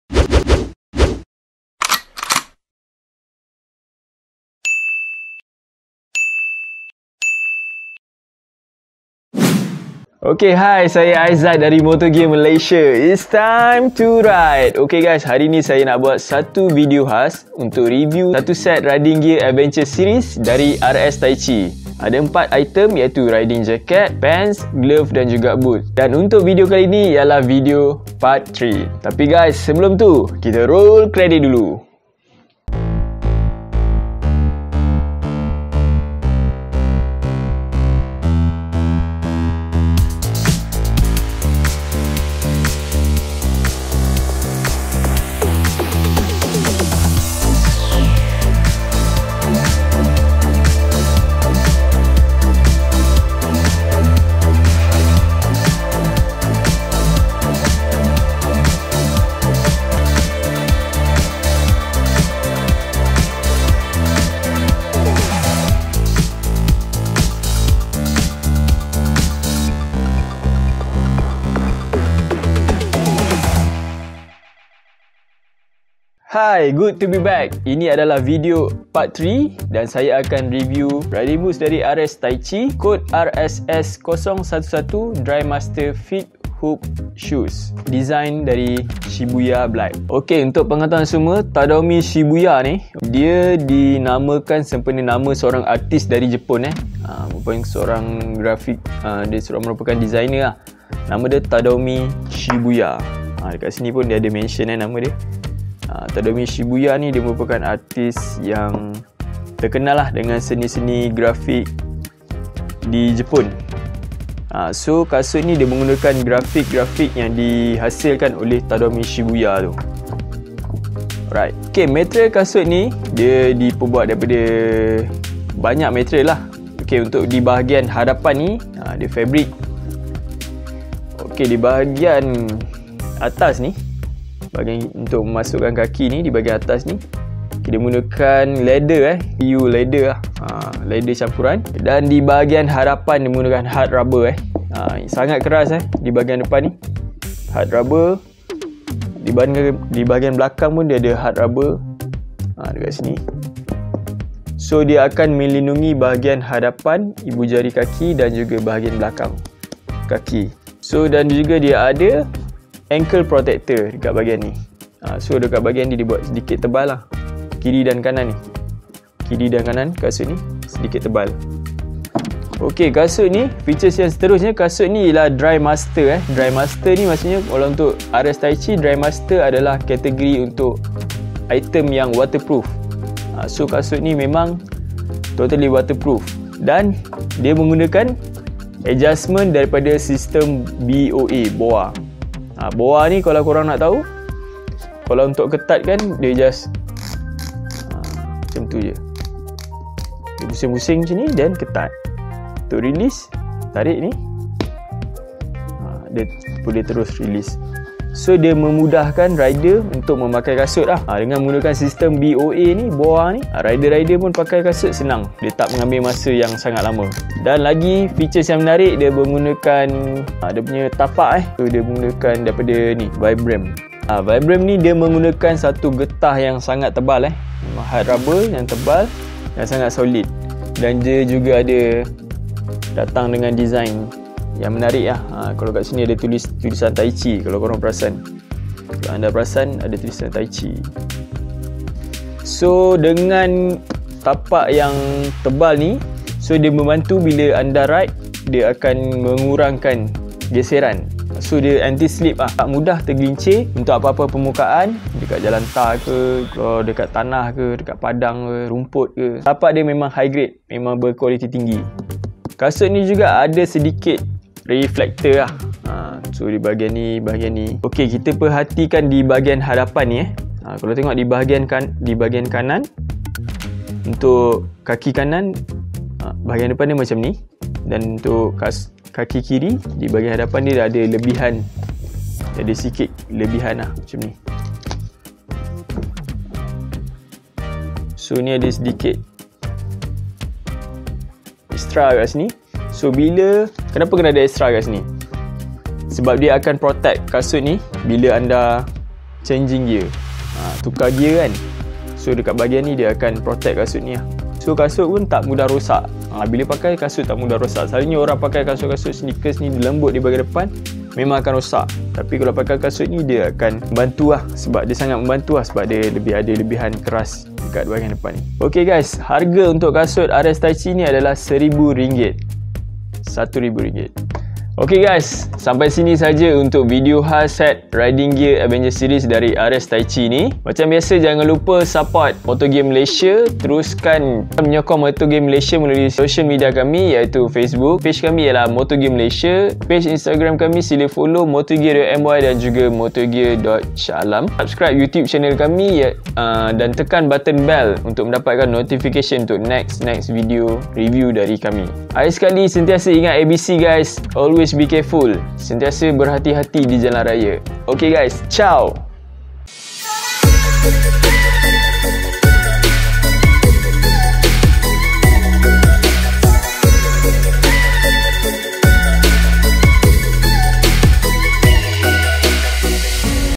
RS TAICHI ADVENTURE SERIES | RSS011 DRYMASTER HOOP SHOES | PART 3 !!!
RSS011 DRYMASTER HOOP SHOES

- Dummy shoe lace
- Molded Protection(heel, ankle, toe)
- Heel guard
- Vibram out sole
- Equipped shift guard for reduce the damage from shift changing. Especially    on the left foot, which is easy to damage, has a large supports for various  pedal operations.
-Equipped reflector for visibility in nighttime. Reflects the headlights of vehicles from behind.
-Equipped hard protection for ankle reduce the impact. Material to avoid friction on inside of heel, reduce damage from friction with the motorcycle.
-Thickness and gentle shape from arch to heel insole reduces fatigue during walking.
BOA fittings system which helps easy adjustment due to dial action.